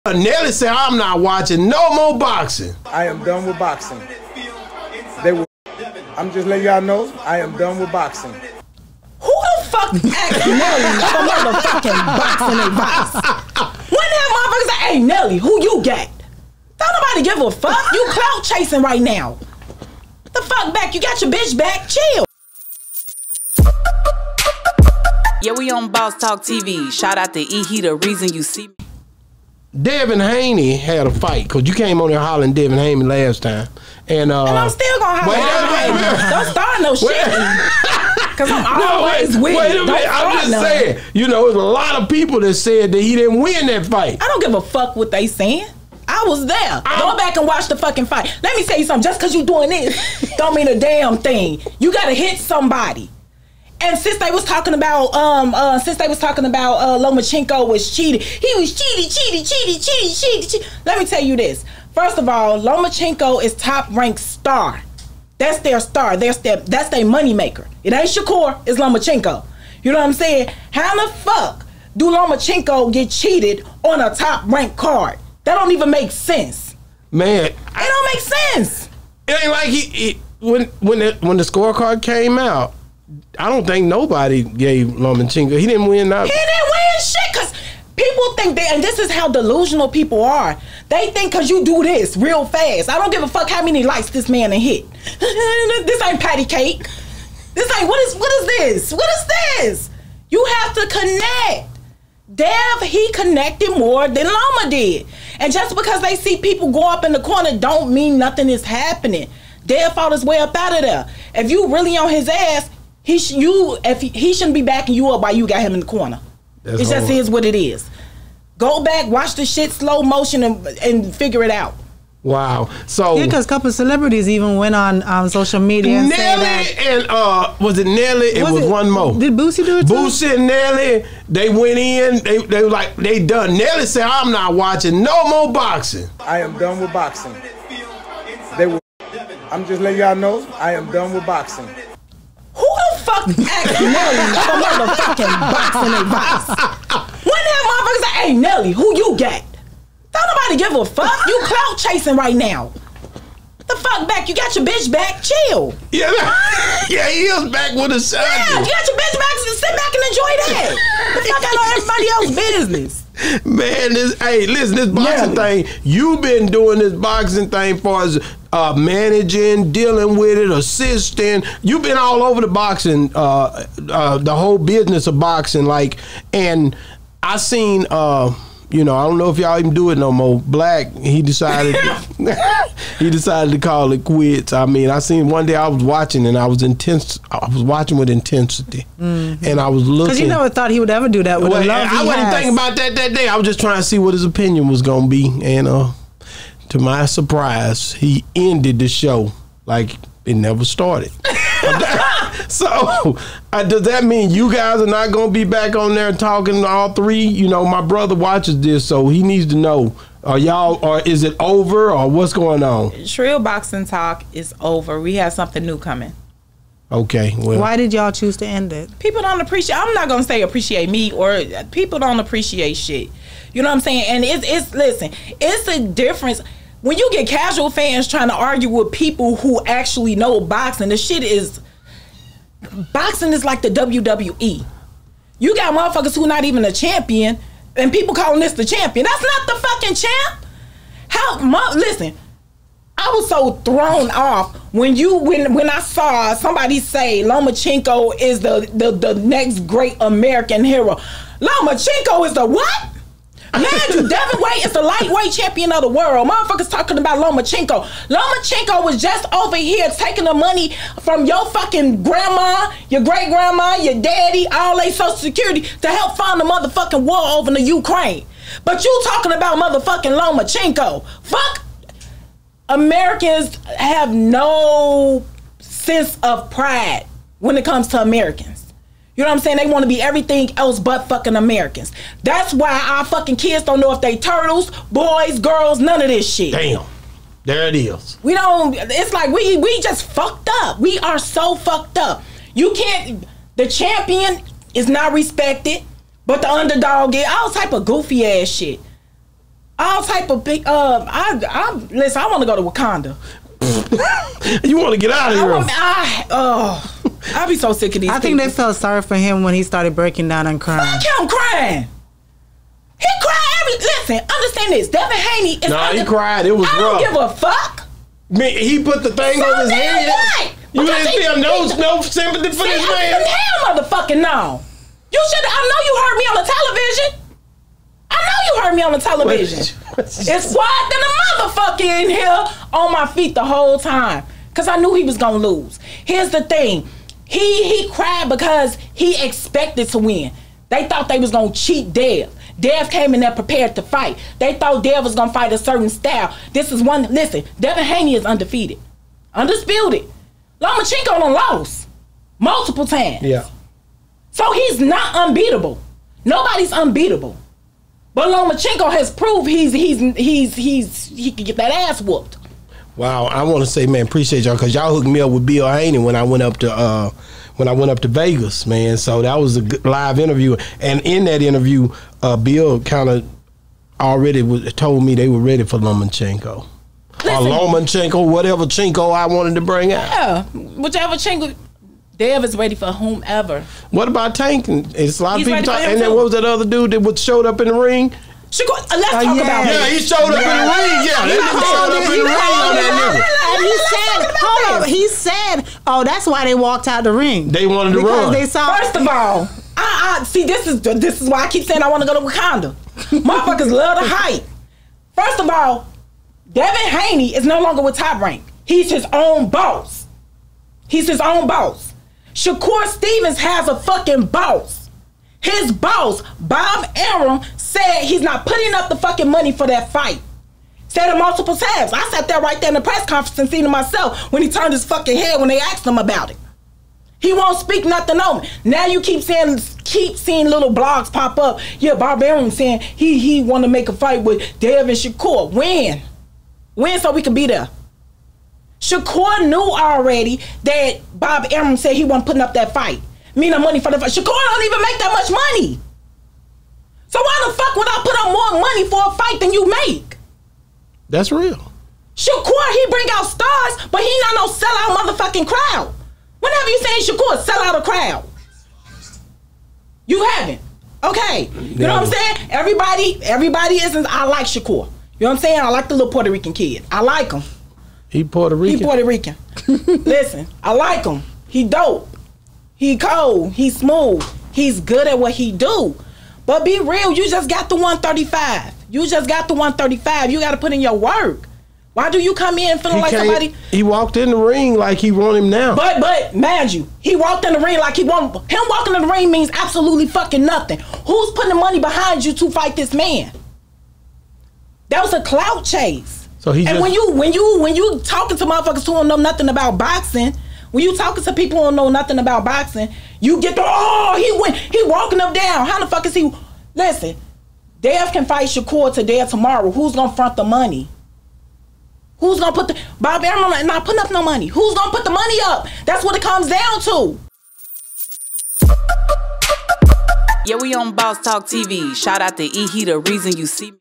CECE REACTS Call Out Nelly For Coming For Devin Haney | Black Shut Down Trill Boxing Talk (Part 2)
Cece Reacts on Boxing!

Devin Haney

Executive Producer: Chasity Swilley
Producer: Stephanie Smith
Video Manager: Dennis Cooper
Content Manager: Shamariah Smith
Public Relations: Tre' Smith